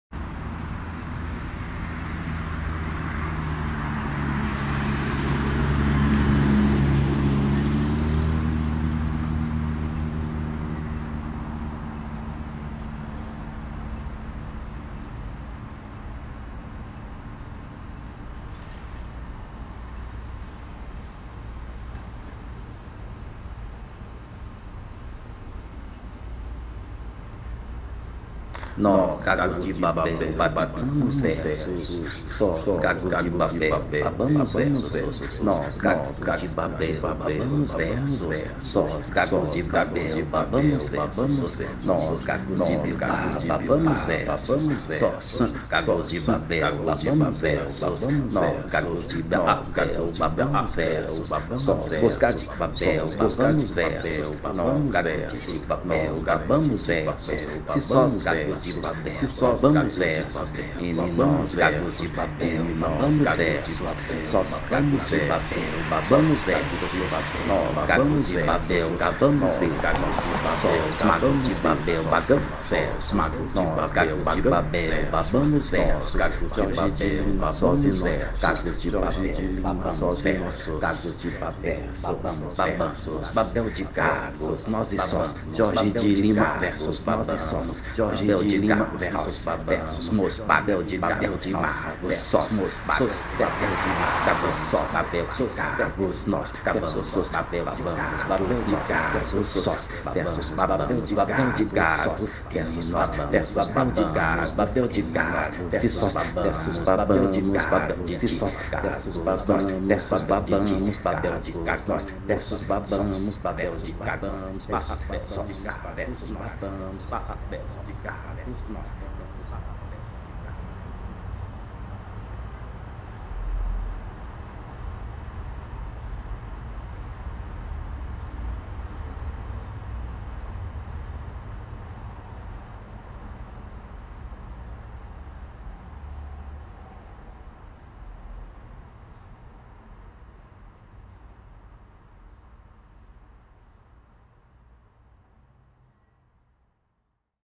15 pedaços de Sossélla S02:EP02: Nós, gagos de babel, bab amos versos, jorge de lima
parte da série 15 PEDAÇOS DE SOSSÉLLA, em homenagem aos 15 anos da morte do poeta Sérgio Rubens Sossélla

NÓS, GAGOS DE BABEL, BAB AMOS VERSOS, JORGE DE LIMA (1996)

nós, gagos de babel, babamos versos, jorge de lima
sós e nós, gagos de babel, babamos, versos

produção: Catatau Filmes
direção: Fernanda Magalhães Ferrari e Gregório Camilo
fotografia: Fernanda Magalhães Ferrari
edição de som e montagem: Gregório Camilo
leitura: Djami Sezostre